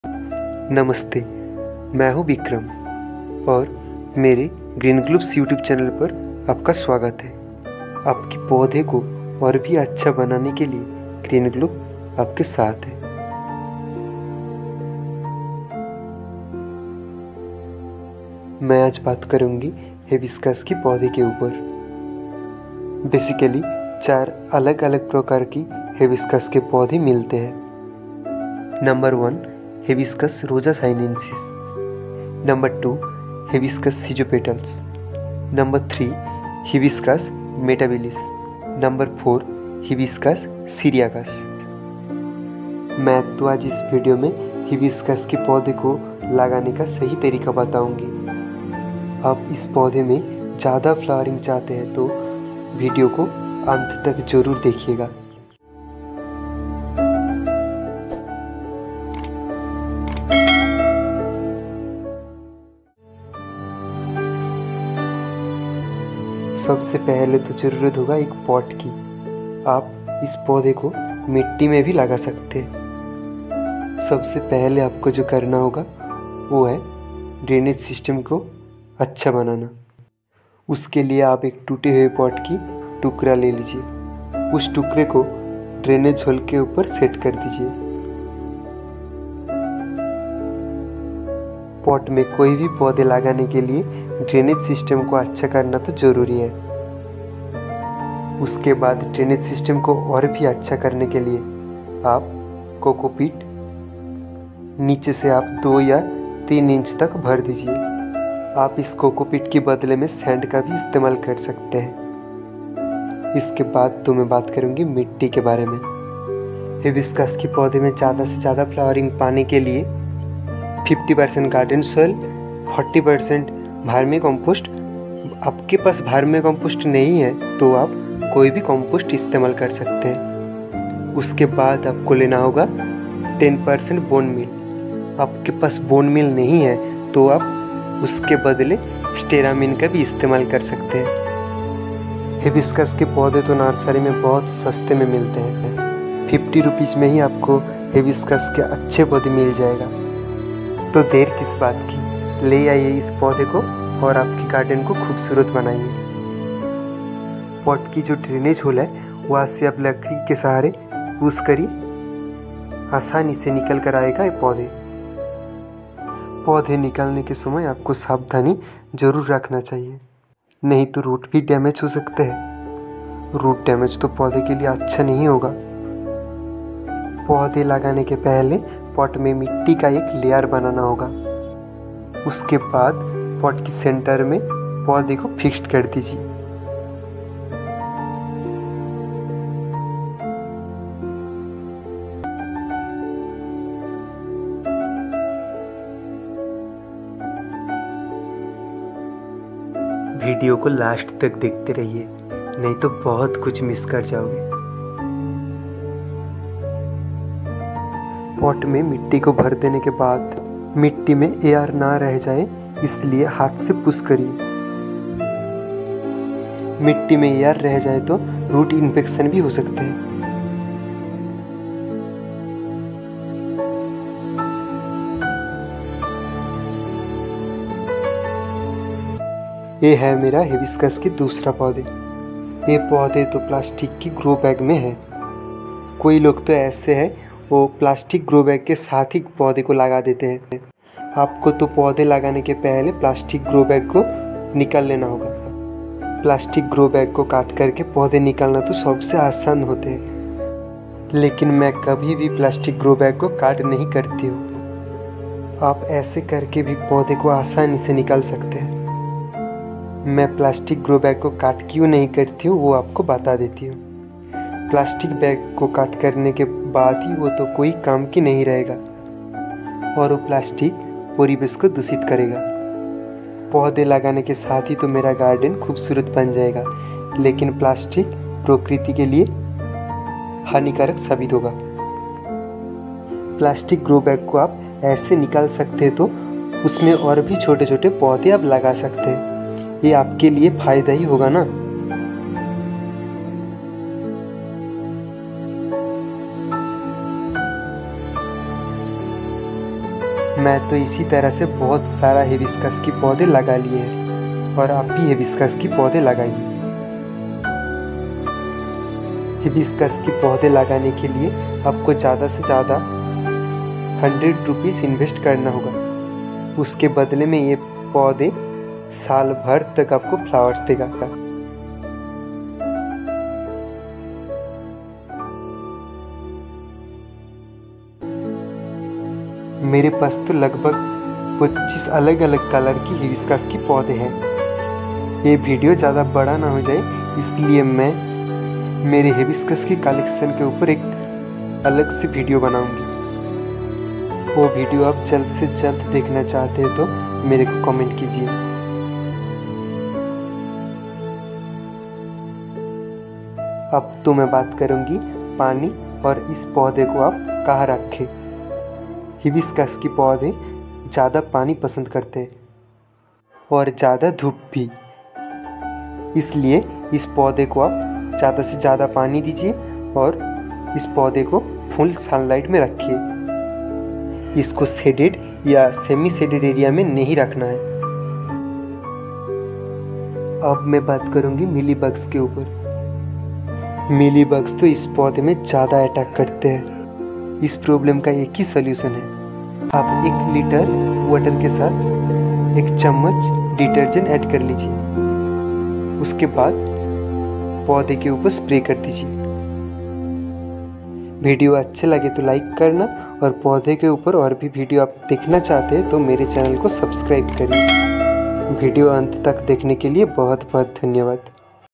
Hibiscus care and reporting || hibiscus ko laganeka sehi tarika || green globe's
Hello friends

In this video I'm going to show you how to repote hibiscus plants. My hibiscus collection get inspired and get one for you.

Four different varieties available in market
1. Hibiscus rosa-sinensis.
2. Hibiscus schizopetalus.
3. Hibiscus mutabilis.
4. Hibiscus Syriacus.

Hibiscus is a flowering plants which is typically grown in warm tropical climates. These flowers are around 200 species of this plant and they belong to the mallow family. Hibiscus is often used for decoration purpose and adds great beauty to the garden.
      A flower quite like no other. Appearing in all her glory in the morning, and then fading as the evening sun caste long shadows amongst the trees.

Hibiscus soil -
1. Garden soil - 50%
2. Vermicompost/compost - 40%
3. Bone meal - 10%

Note- This video only provides education knowledge to gardener friends and the information described "in this video has shared my knowledge"